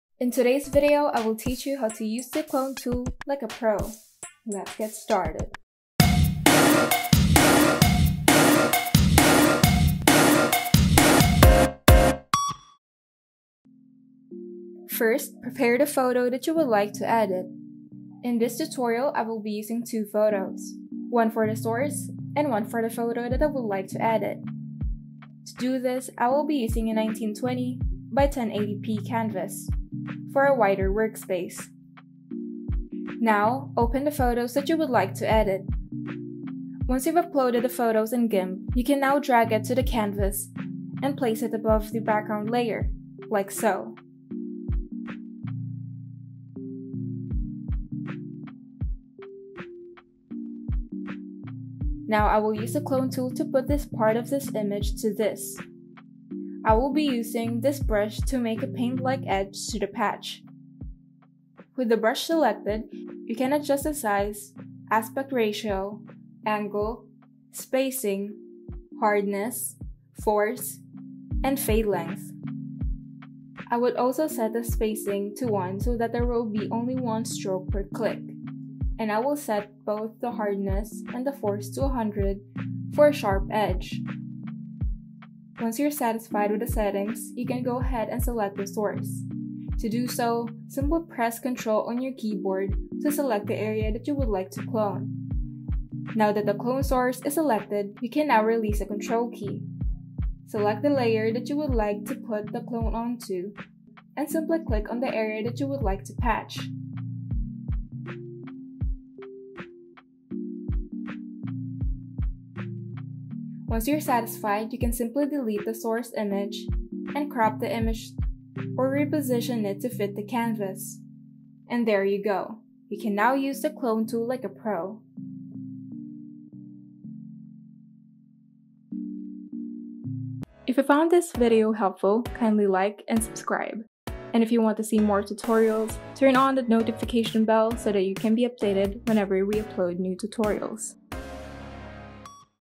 GIMP - How to Use the CLONE TOOL Like a PRO!
The clone tool may be one of the simplest ways to create cool effects in Gimp.  If you don't have much experience playing with this tool, I encourage you to try it out and experiment with it once you watch this tutorial.  Just have some fun with it!

Don't have Gimp yet?